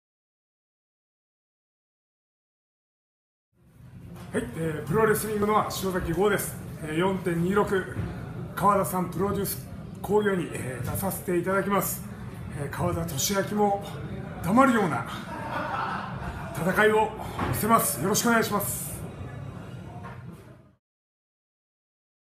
川田利明プロデュース「HOLY WAR〜序章〜」潮崎豪選手からのコメント
川田利明プロデュース「HOLY WAR〜序章〜」
潮崎豪選手からのコメント

【メインイベント】
6人タッグマッチ 60分1本勝負

越中詩郎(フリー)＆藤原喜明(藤原組)＆本田多聞(フリー)
VS
潮崎豪(NOAH)＆清宮海斗(NOAH)＆樋口和貞(DDT)